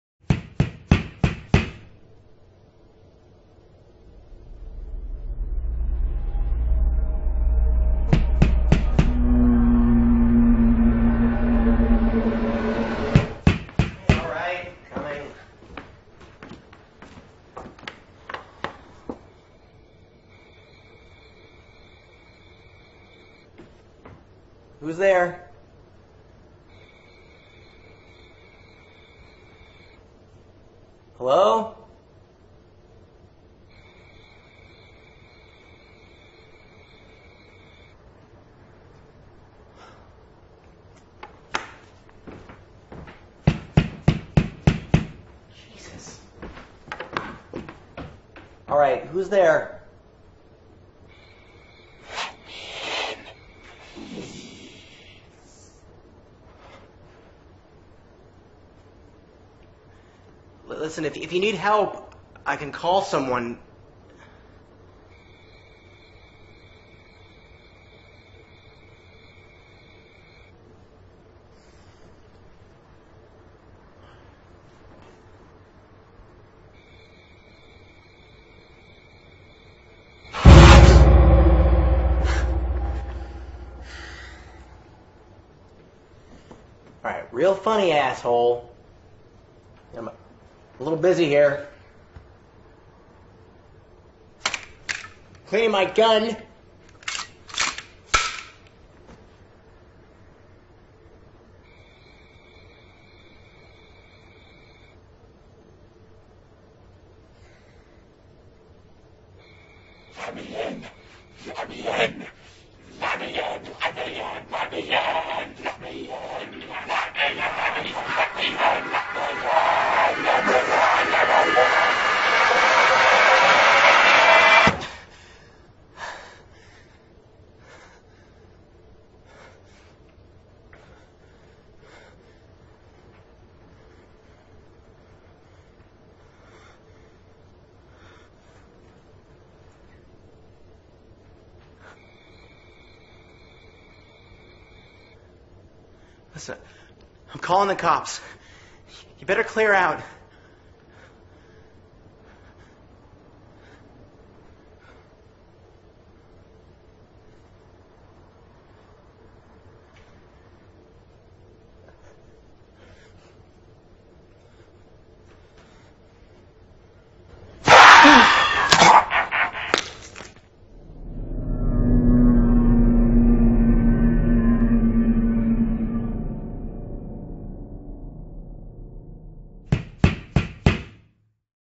SFM| Tails Doll| RASP| Short Horror
No podia dejar pasar las fechas del horror para hacer un short horror film animado